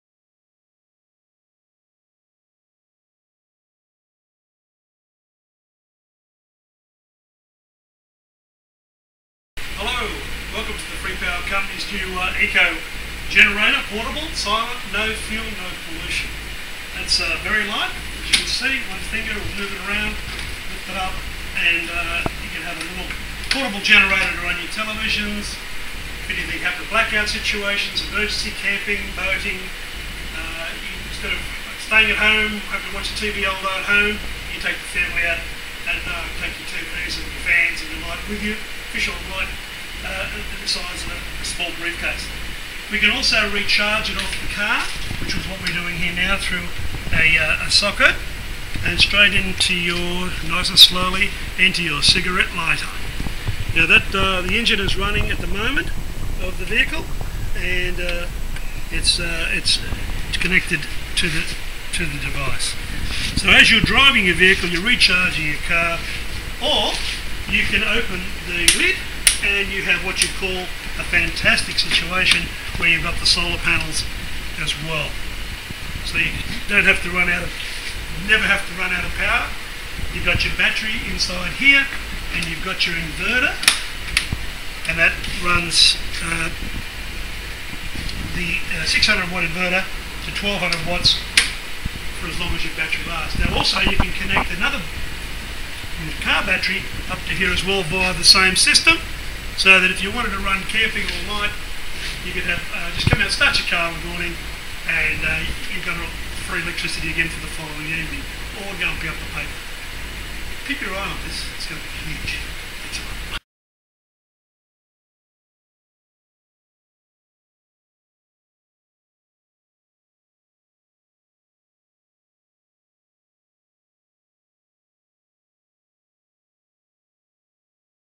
FREE ELECTRICITY - ECO GENERATOR CAR RECHARGING
Here is 600 watt - 1200 watt Eco Generator that is able to be recharged via a car cigarettel lighter socket!

Recharge it while you drive!

The Eco Generator is a portable, silent, eco friendly electricity generator needing no fuel, produces no emissions and can be recharged by solar panels, portable wind generators, battery rechaging via grid connected electricity.

The Eco Generator has many great uses, including on work sites, where you can recharge drills, sanders, nail guns, jack hammers and other equipment.

The 600 watt - 1200 watt Eco Generator is also useful for households, units and condos, powering up TV's, DVD's, Radios, Stereos, Computers, Lap Tops, Phone Chargers, Lights, Lamps and other household appliances.

The Eco Generator can also be used in third world nation villages, huts and dwellings, for emergency portable electricity in disaster relief situations, events, festivals and remote gatherings.

The Eco Generator is perfect for camping, caravanning, fishing, boating and general recreational use.

The 600 watt - 1200 watt Eco Generator retails for $!995.00 excluding deiivery.